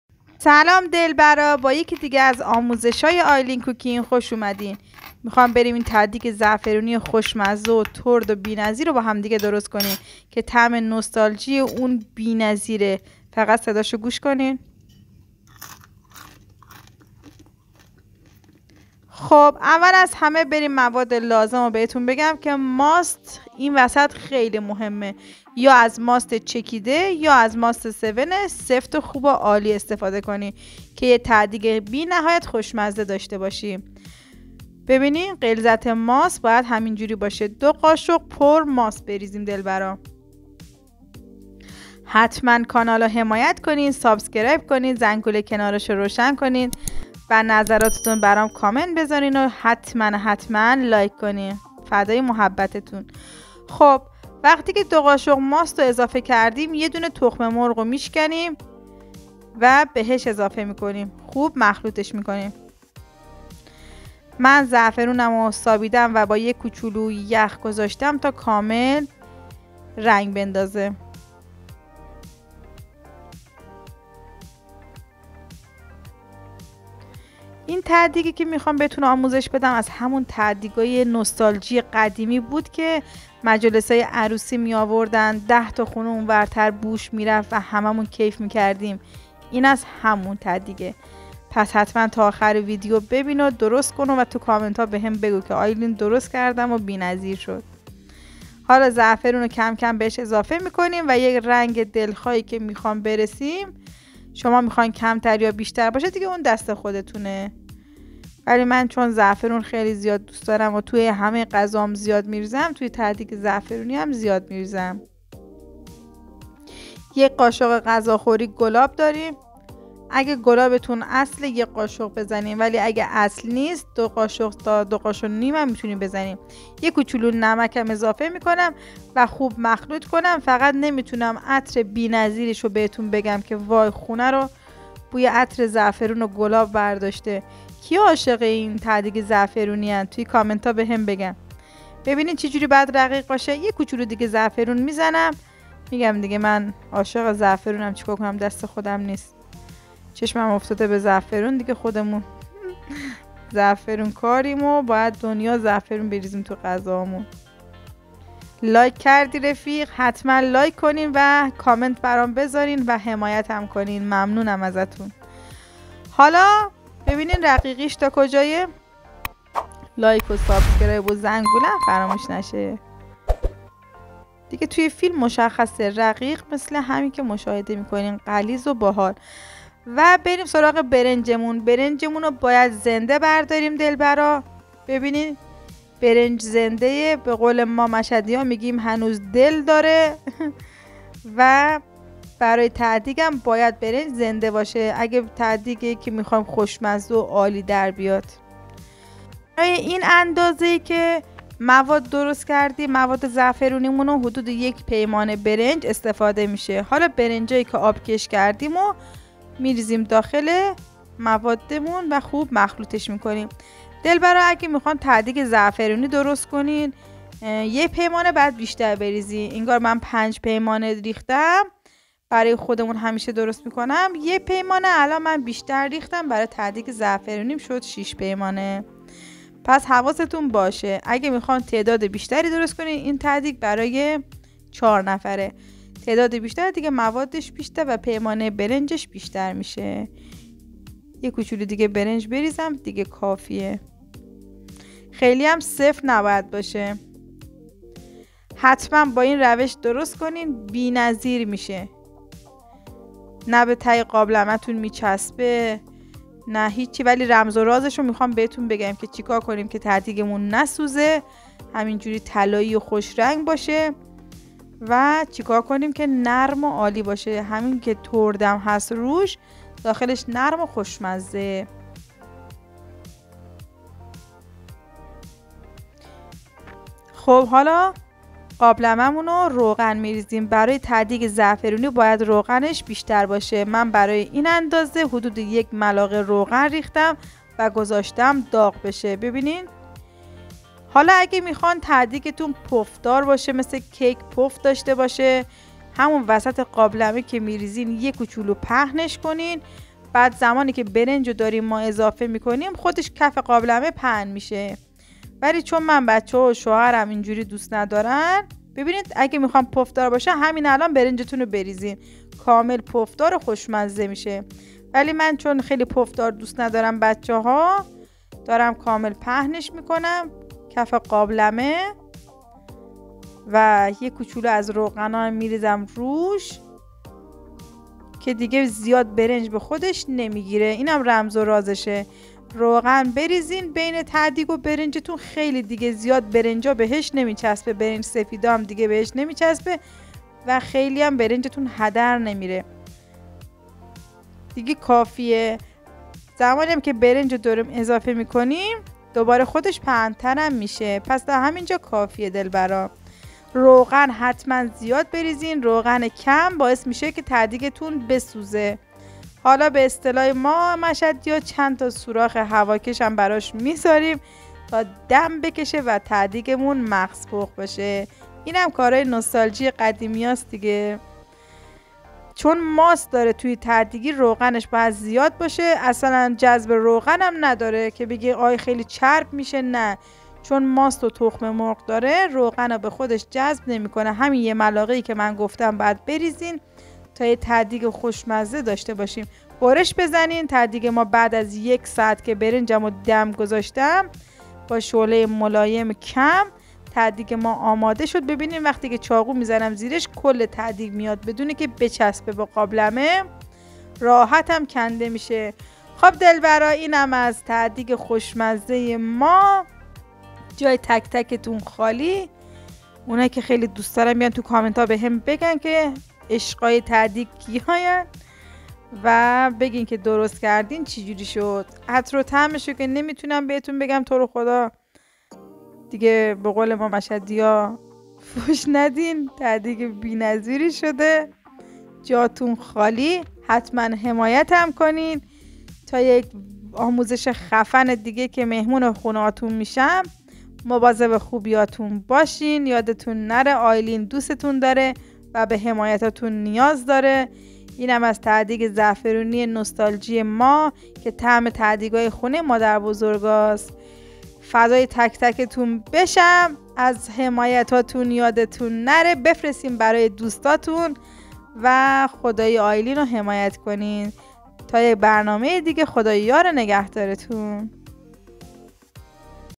ته دیگ زعفرانی مجلسی:ته دیگ زعفرانی نوستالژی مادربزرگا
سلام دلبرااااا خوبین خوشین امروز آموزش خوشمزه داریم ته دیگ زعفرانی مجلسی نوستالژی لایک کامنت سابسکرایب زنگوله بکوب عزیزم بریم آموزش ببین💋💋💋💋❤️❤️❤️❤️🙏🏾🙏🏾🙏🏾🙏🏾👍👍👍👍
موادلازم ته دیگ زعفرانی مجلسی
ماسک سفت :۲قاشق
زعفران دم کرده:به مقدار لازم
تخم‌مرغ:۱عدد
نمک:کمی
گلاب :۱قاشق غذاخوری